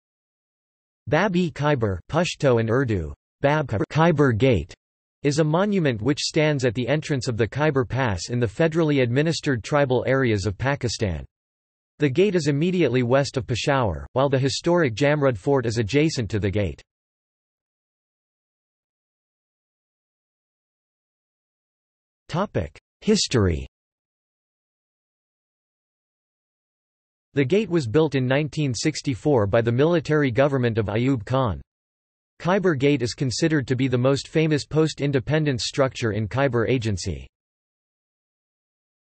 Bab-e-Khyber | Wikipedia audio article
This is an audio version of the Wikipedia Article:
Bab-e-Khyber

00:00:18 undefined

- Socrates

SUMMARY
Bab-e-Khyber (Pashto and Urdu: باب خیبر‎; "Khyber Gate") is a monument which stands at the entrance  of the Khyber Pass in the Federally Administered Tribal Areas of Pakistan. The gate is immediately west of Peshawar, while the historic Jamrud Fort is adjacent to the gate.